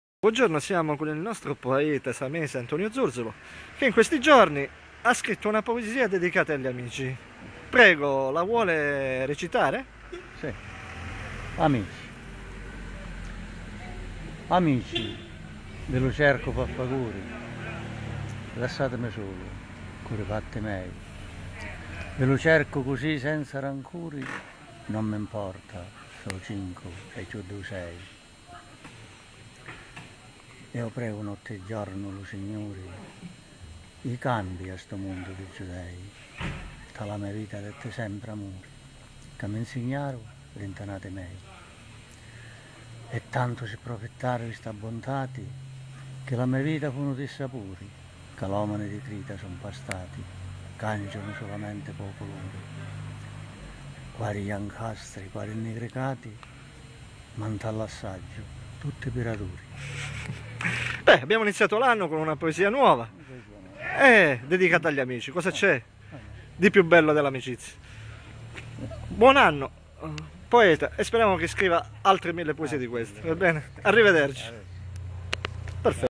IL POETA SAMESE ZURZOLO RECITA UNA NUOVA POESIA
ANNO NUOVO POESIE NUOVE!
Il poeta samese Antonio zurzolo recita la sua nuova poesia "AMICI" scritta nelle festività natalize.